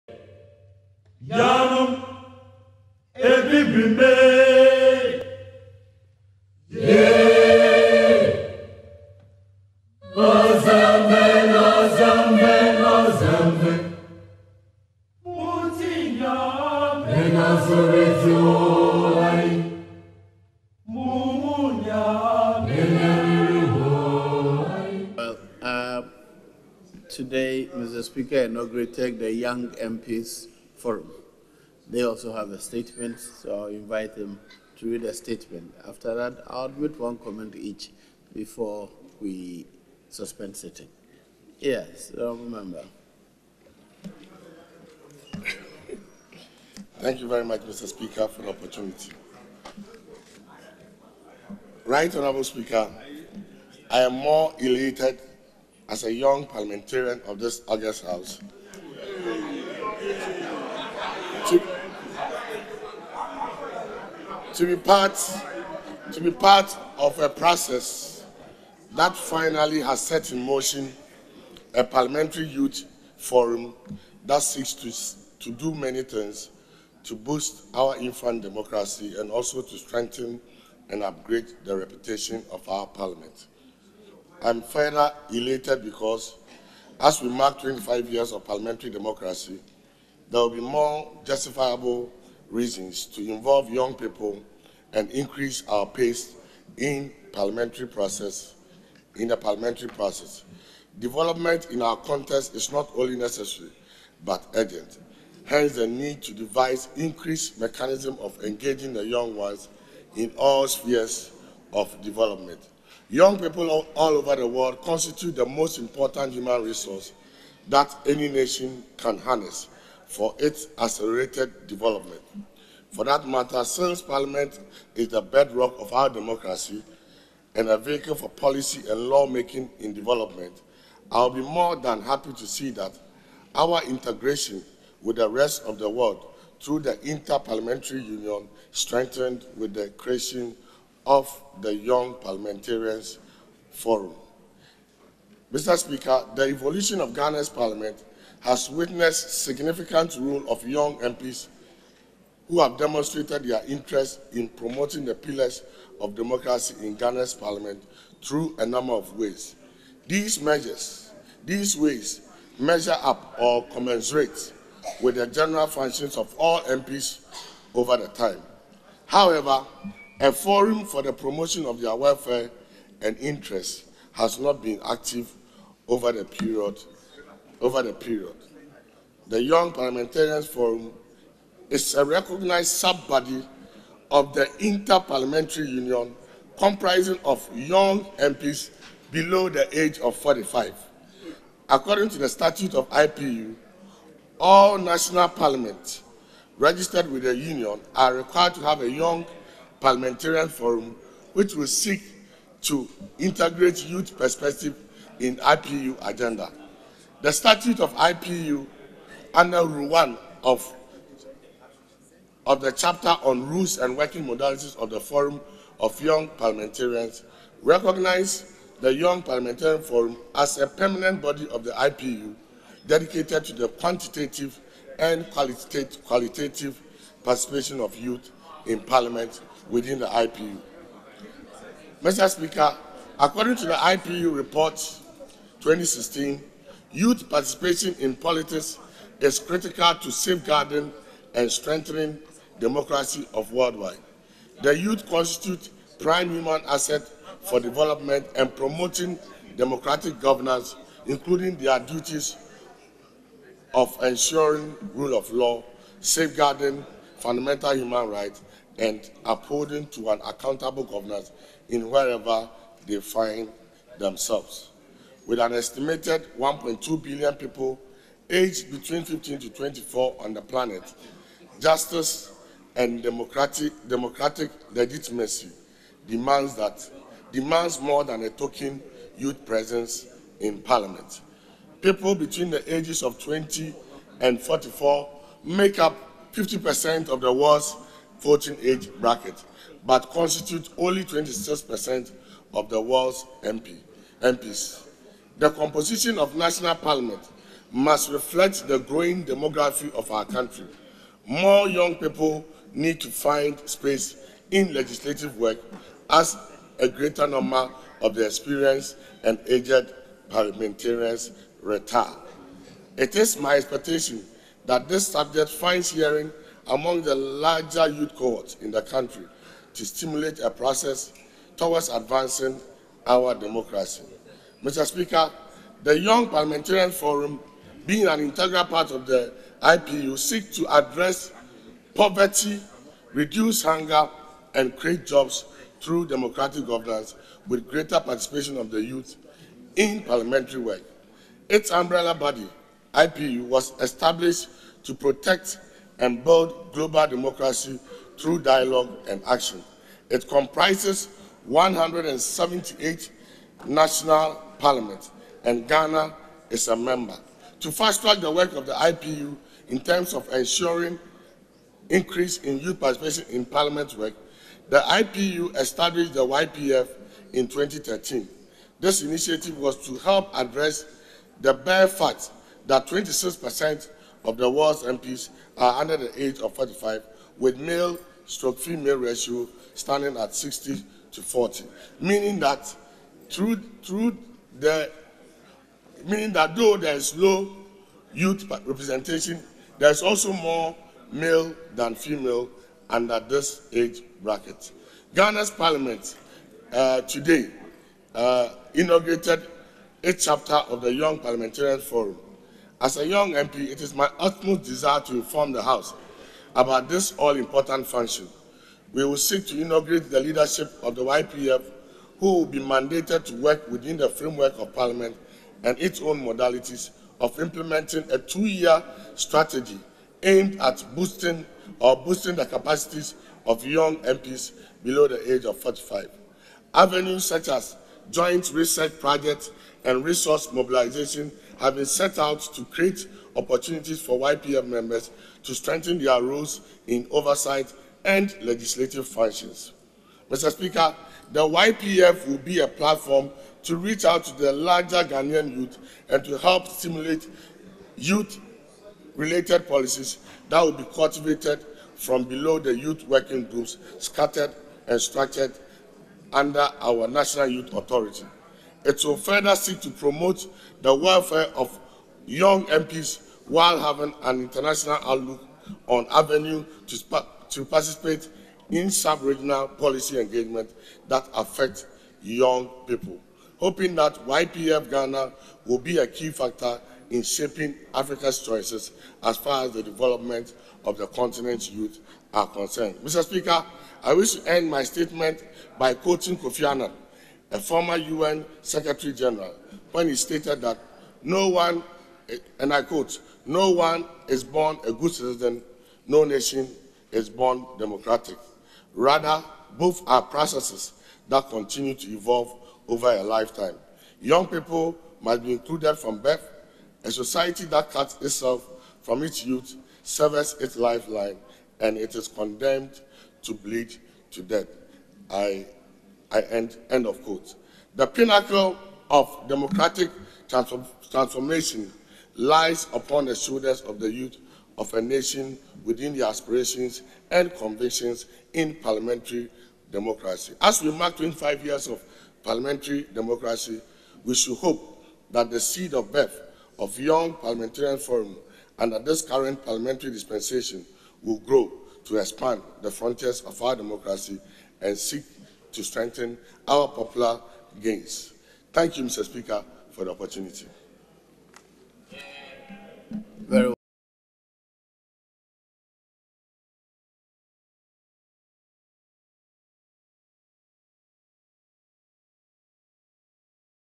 HON. ASEMANYI KOJO MP. GOMOA EAST
25th July 2018 Statement on the independence celebration of the state of Israel.